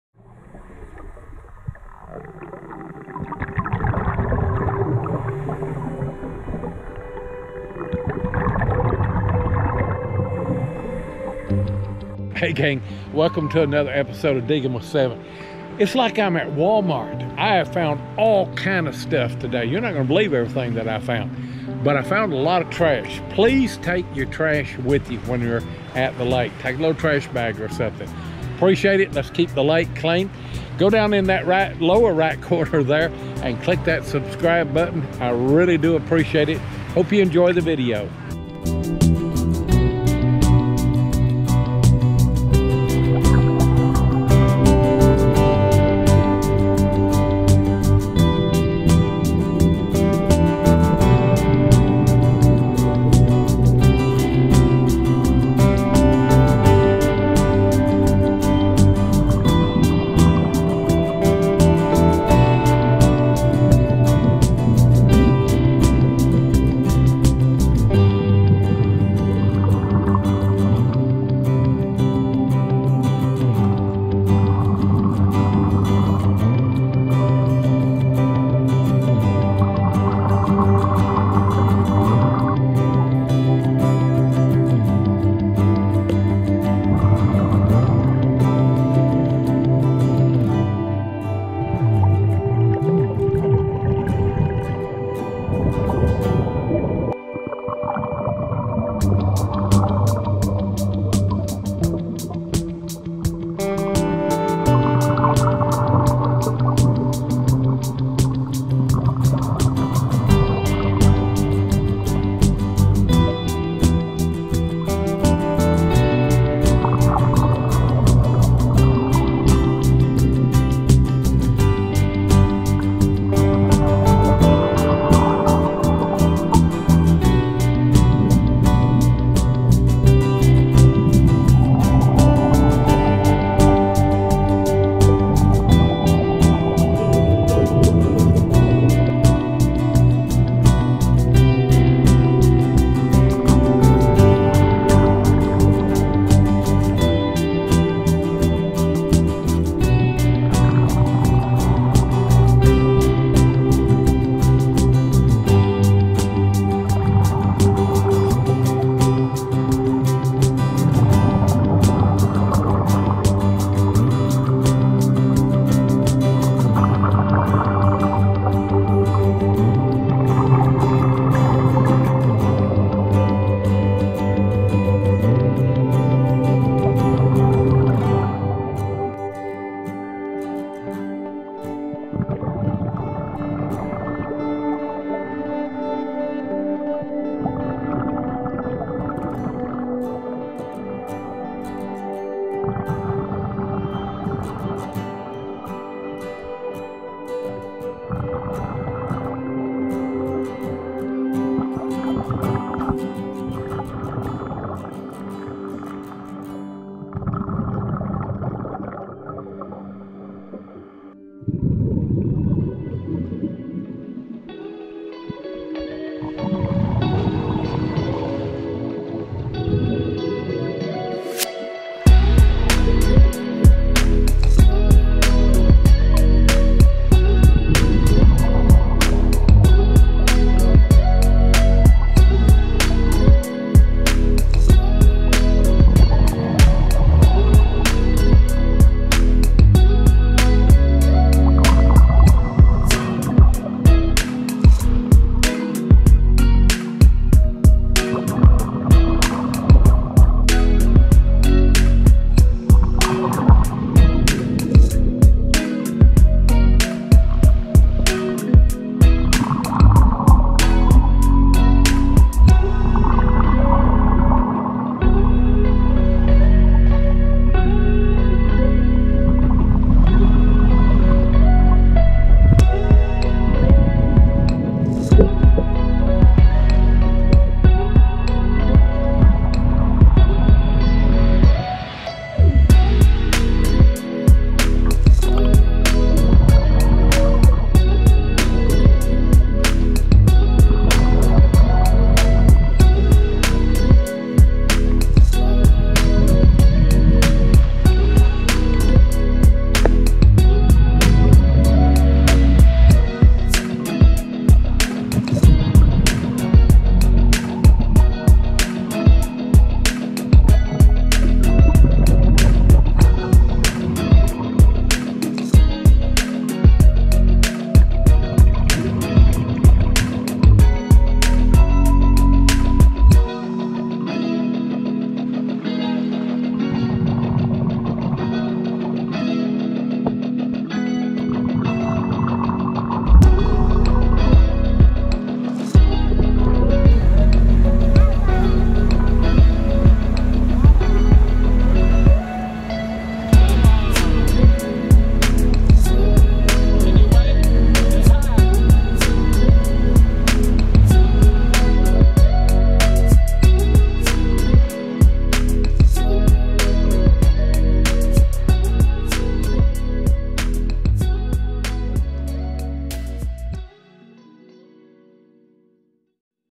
I hit the JACKPOT on FINDS scuba diving… It’s like WALMART down there! – Episode 313
I hit the JACKPOT on FINDS scuba diving the bottom of Dale Hollow Lake… it’s like WALMART down there! I find a little bit of EVERYTHING! I also see a lot of nice fish in the process. It was a great dive!
But I also cleanup a lot of trash people have thrown into the lake. Come on folks… carry a trash bag with you! Don’t throw your trash in the lake!